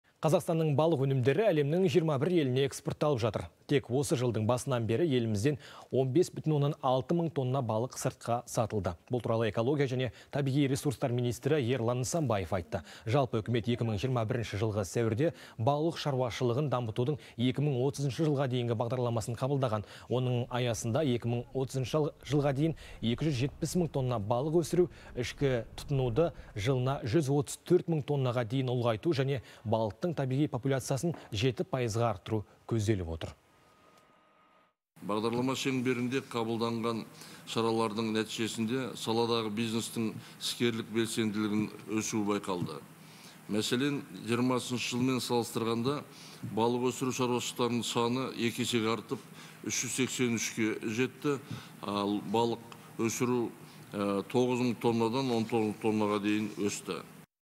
Балық өнімдері әлемнің 21 еліне экспортталып жатыр
Қазақстанның балық өнімдері әлемнің 21 еліне экспортталып жатыр. Тек осы жылдың басынан бері елімізден 15,6 мың тонна балық сыртқа сатылды. Бұл туралы экология және табиғи ресурстар министрі Ерлан Нысанбаев айтты.

Жалпы Үкімет 2021 жылғы сәуірде балық шаруашылығын дамытудың 2030 жылға дейінгі бағдарламасын қабылдаған. Оның аясында 2030 жылға қарай 270 мың тонна балық өсіру, ішкі тұтынуды жылына 134 мың тоннаға дейін ұлғайту және балықтың табиғи популяциясын 7 пайызға арттыру көзделіп отыр.